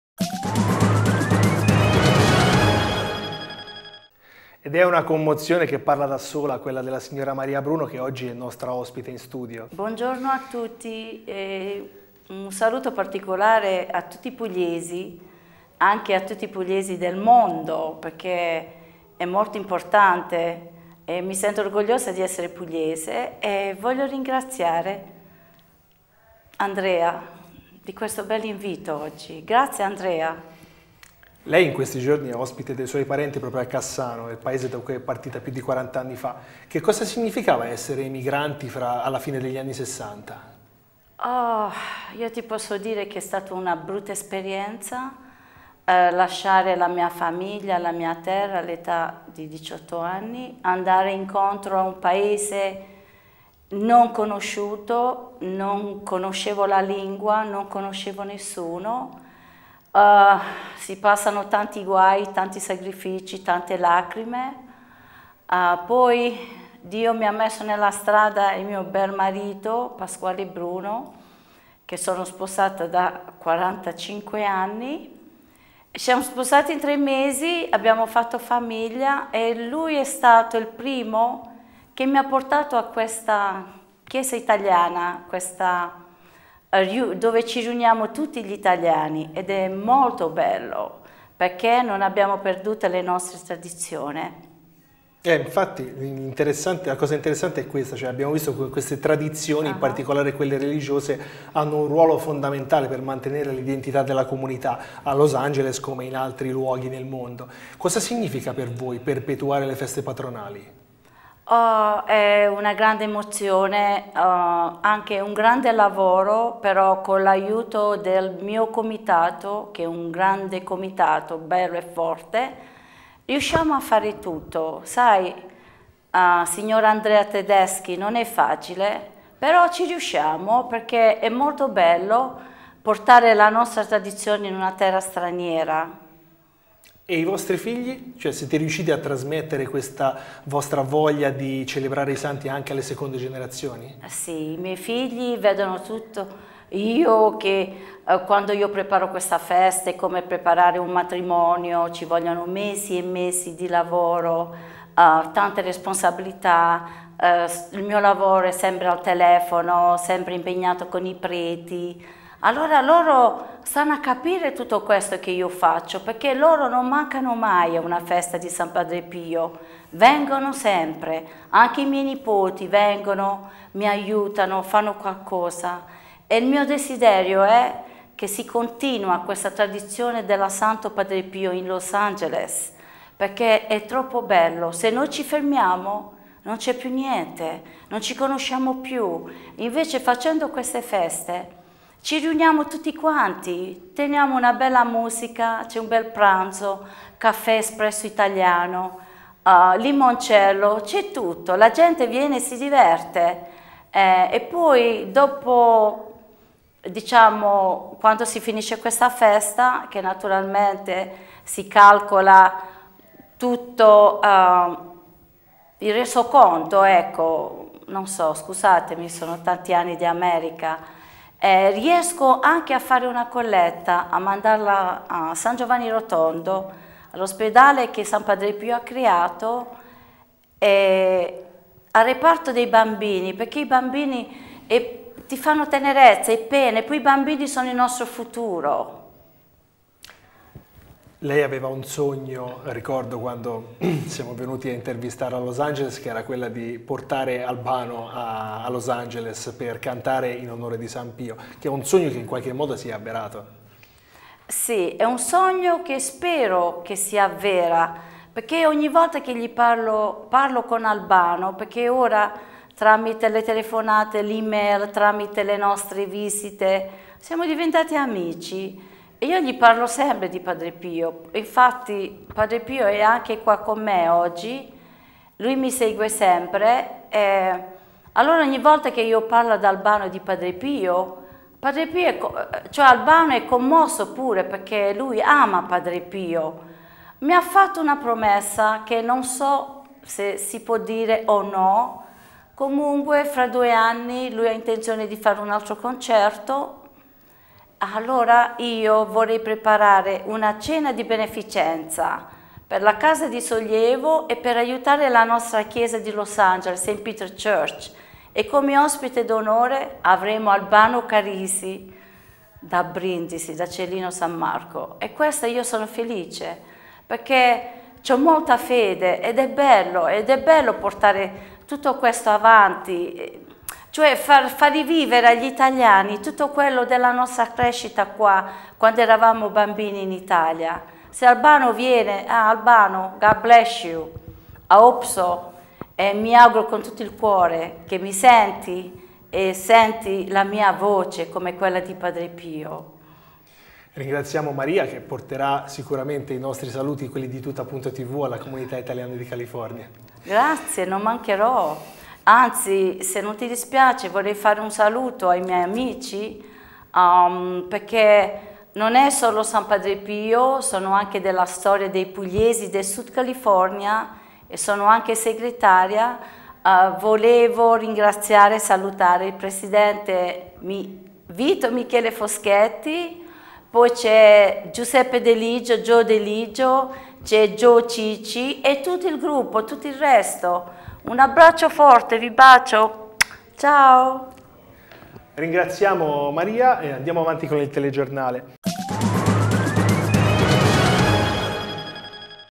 Da Los Angeles alla Puglia, nel segno di Padre Pio
Una commozione che parla da sola quella della signora Maria Bruno, oggi ospite del TG di PuntoTV per raccontare la sua fede per Padre Pio, che ha rinsaldato il suo legame con il suo paese d'origine, Cassano delle Murge, da cui è partita più di 40 anni fa.